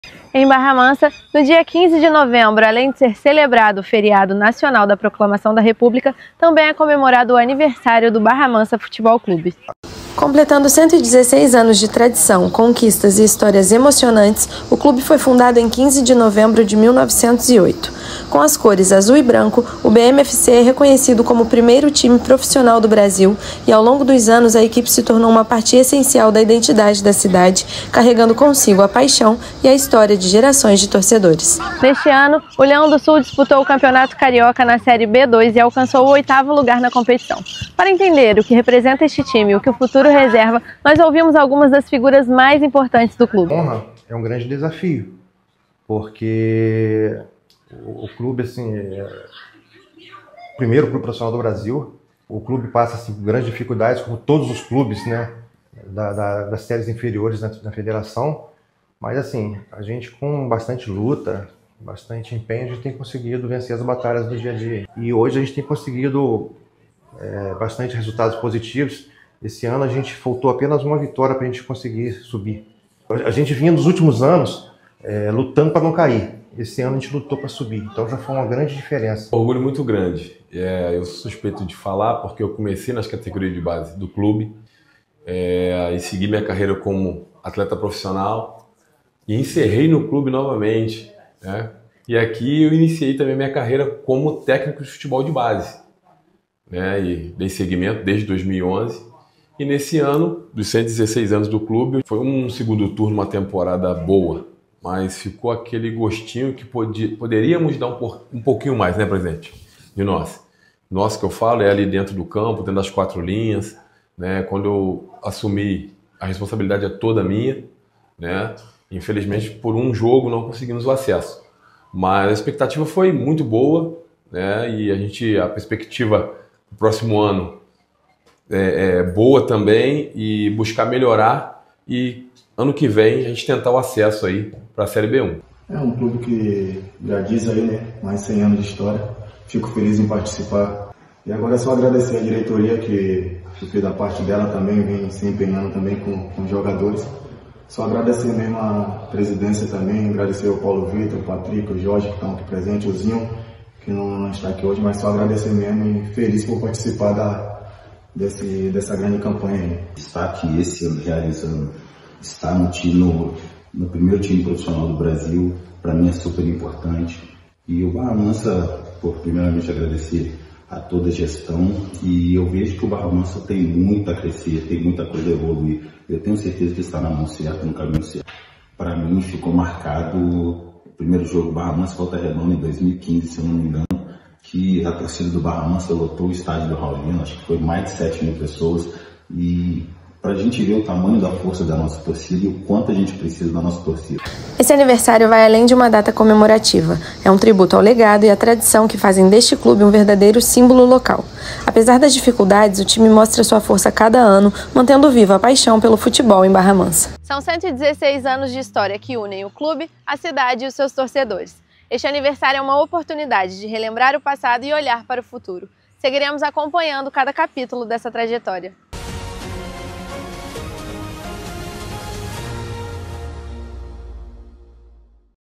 O Barra Mansa Futebol Clube completa 116 anos neste 15 de novembro
Acompanhe na reportagem a trajetória do clube, os desafios enfrentados ao longo dos anos, as perspectivas para o futuro e o legado que mantém viva a paixão pelo Leão do Sul!